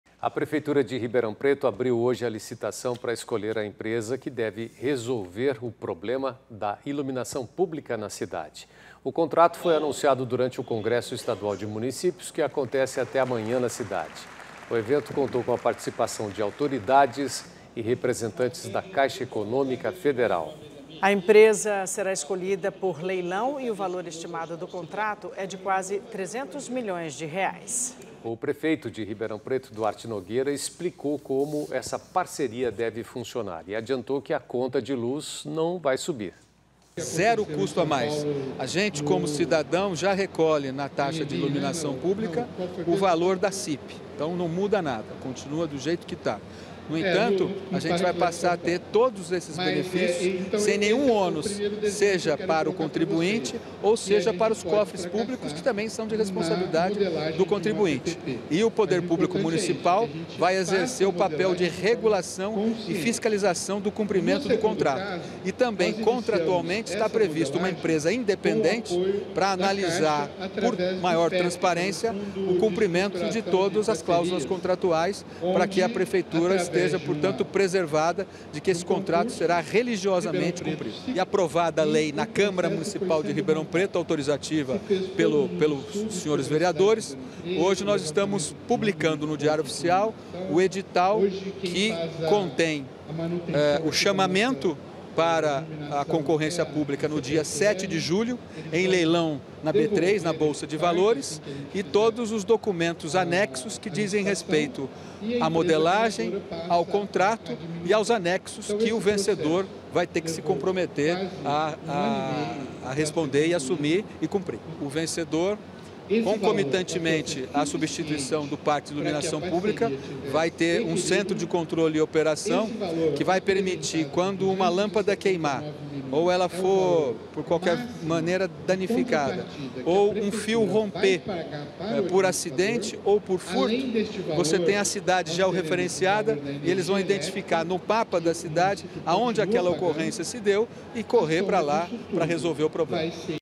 Parceria público-privada: Prefeitura abre ‘licitação da iluminação pública’ em Ribeirão Preto
A Prefeitura de Ribeirão Preto abriu nesta quarta-feira (10) a licitação para escolher a empresa que deve resolver o problema de falta de iluminação pública na cidade. A empresa será escolhida por leilão e o valor estimado do contrato é de quase R$ 300 milhões.